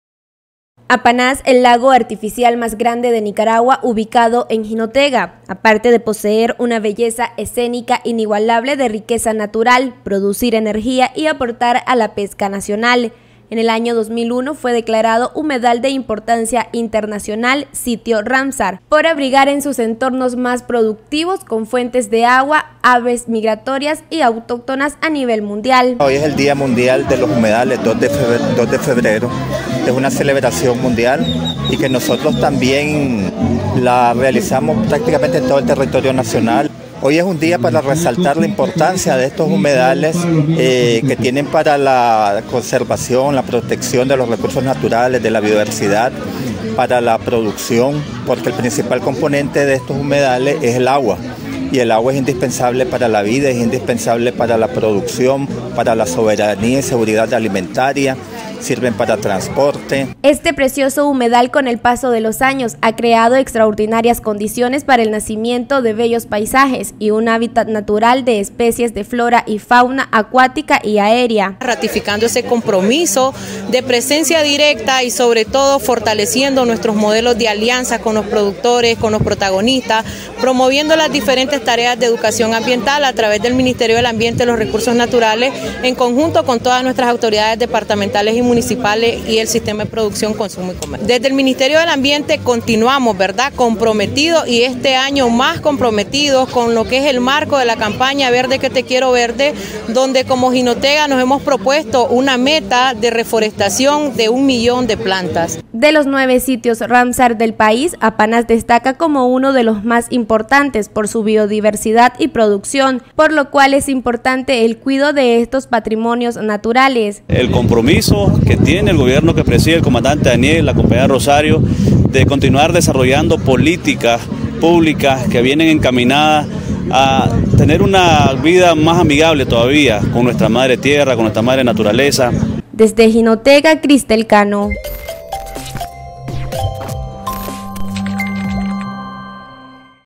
Apanás, uno de los 9 sitios ramsar de Nicaragua
Es el primer lago artificial de Nicaragua creado en 1964 por la inundación del valle de Apanás con el caudal del río Tuma, Jigüina, Mancotal y San Gabriel.
Y su nombre significa “Vado de Agua”